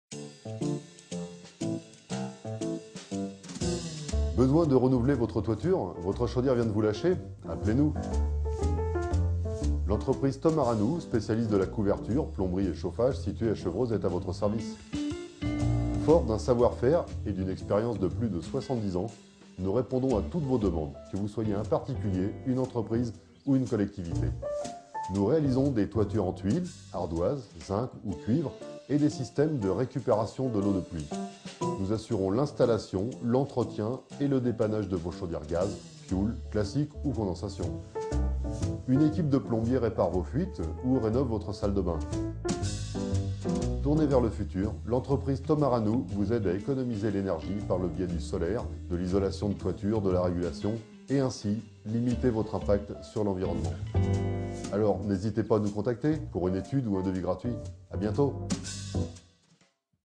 Thomas Rannou situé à Chevreuse dans les Yvelines 78 couverture plomberie zinguerie chauffage
Thomas Rannou est à votre service depuis quarante ans à Chevreuse dans le département des Yvelines. Chez nous, vous trouvez un accueil chaleureux, un savoir-faire, une qualité de travail reconnue. Nous sommes spécialisés dans l'installation et le dépannage de chauffages, la couverture, l'installation d'énergies renouvelables ainsi que dans la plomberie. Vous avez la possibilité de venir voir nos catalogues et nos modèles de produits. Nous vous assurons un travail sérieux et soigné qui répond à vos moindres attentes. Nous sommes à votre entière écoute et vous conseillons comme il se doit dans le choix de vos projets.